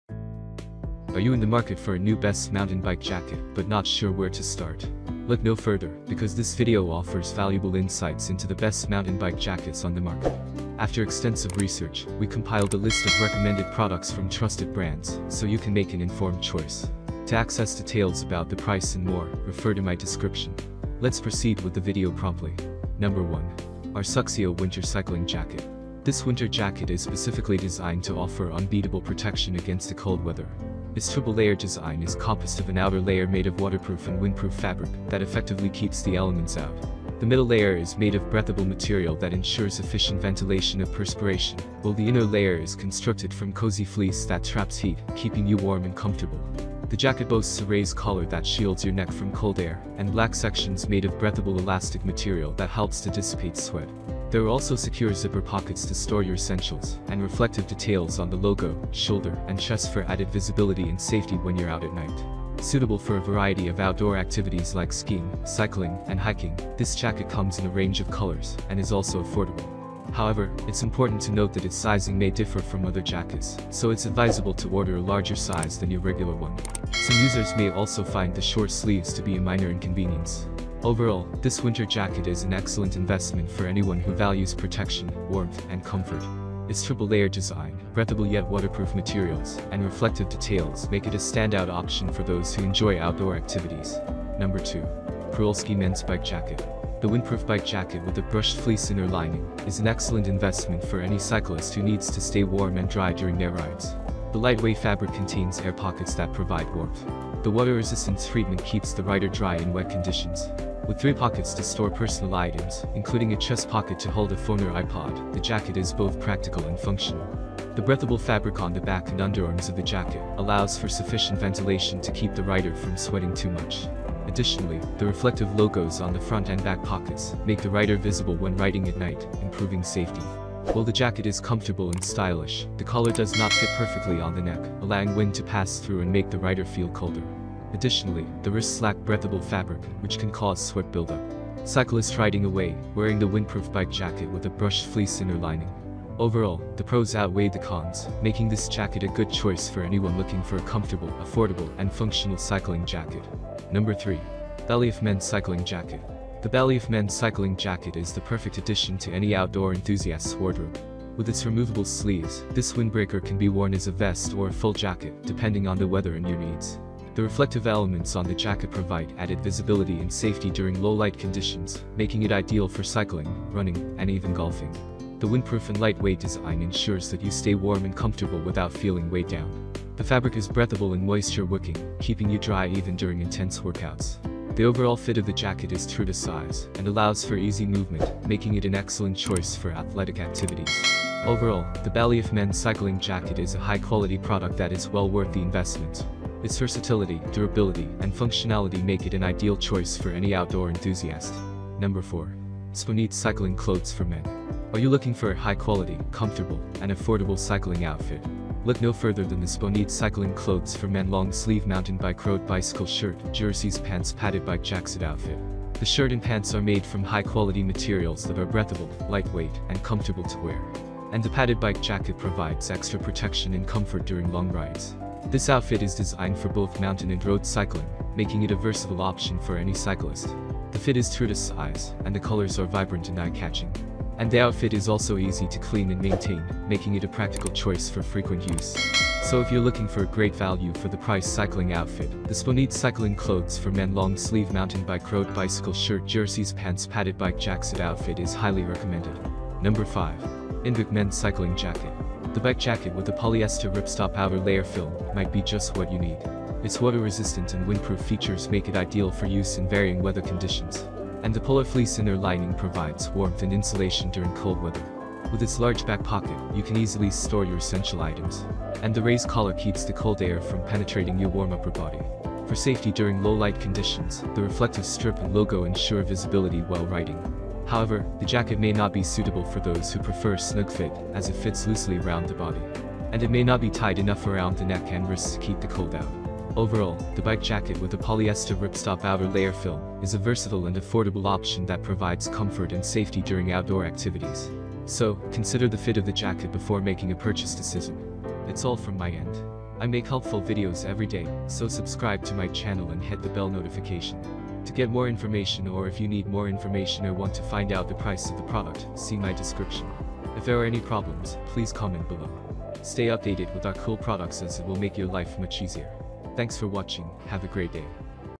Best Mountain Bike Jackets For The Money 2023 | Top 5 Best Mountain Bike Jackets Review
~ Product Link ~
►1. Arsuxeo winter cycling jacket
►2. Przewalski Men’s Bike Jacket
►3. BALEAF Men's Cycling Jacket
►4. sponeed Cycling Clothes for Men
►5. Inbike Men’s Cycling Jacket